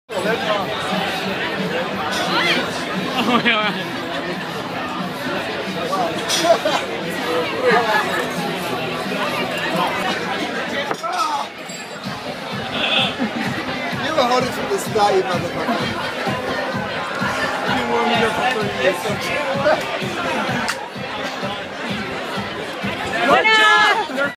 Chef Curtis Stone VS Chef Emmanuel Delcour
The Ultimate Chef Arm-Wrestling Battle @ The LA Food n' Wine Festival 2013